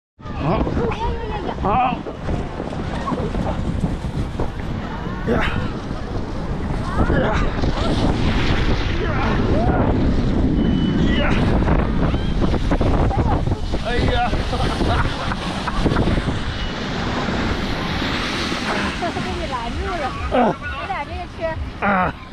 Shenyang Olympic Sports Center
Shenyang Olympic Sports Center's International Ice and Snow Carnival offers a variety of rides and games suitable for tourists of different ages. Visitors will enjoy themselves here from dawn to dusk. People experiencing snowy mountain surfing in the center will slide hand in hand together, and feel a rush of adrenaline when rushing down the slope. The ice slide is 12 meters high and 20 meters long. Sitting in a basket and going down the ice slide, visitors feel like they are back to the winter camp they expected most as children.